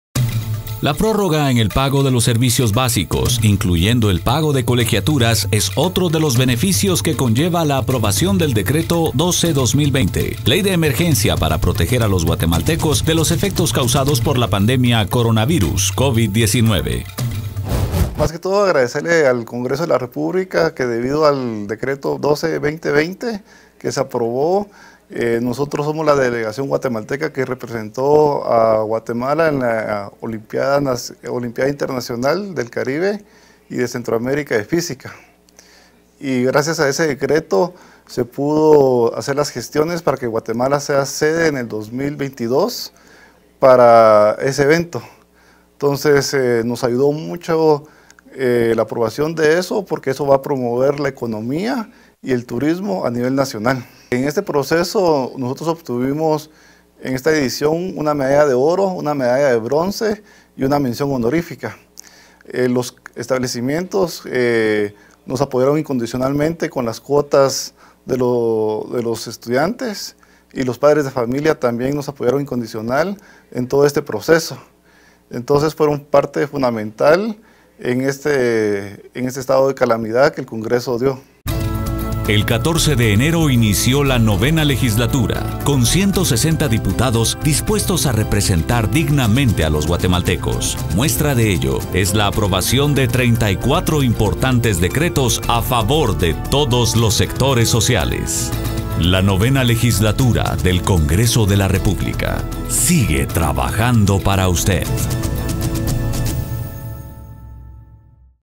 Resultados2020, Decreto 12-2020
La prórroga en el pago de los servicios básicos, incluyendo el pago de colegiaturas, es otra de las bondades del Decreto 12-2020, a continuación, profesores y estudiantes narran los beneficios que obtuvieron.